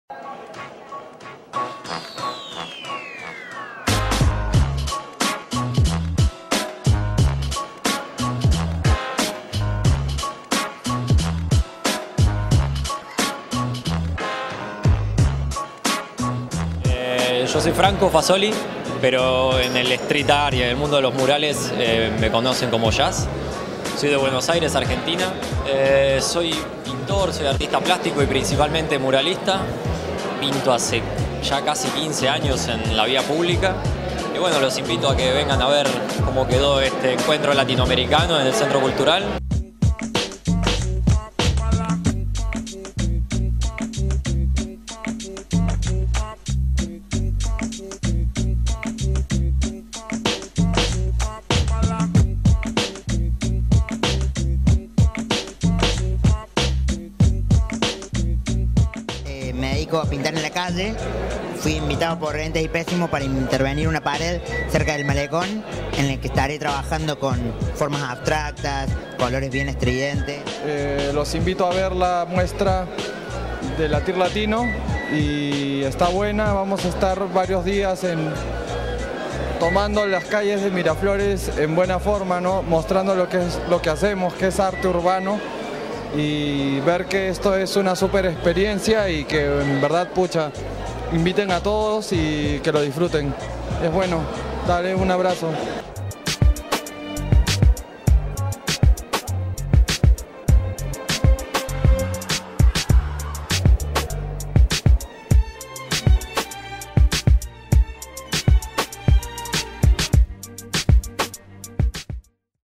Festival Latir Latino en Miraflores
Los artistas del Festival Latir Latino expusieron algunos de sus trabajos en la Galería Raúl Porras Barnechea de Miraflores del Centro Cultural Ricardo Palma. Latir Latino es el primer festival latinoamericano de arte en la calle. Del 25 de febrero al 11 de marzo, el arte tomará determinadas calles y espacios de nuestro para dar paso a originales intervenciones de graffiti de artistas de Perú, Argentina, Colombia, Paraguay, Chile y Puerto Rico. El festival busca convertir a Miraflores en la galería de arte más grande de sudamérica, donde el público no solo podrá apreciar las obras sino también su proceso de creación.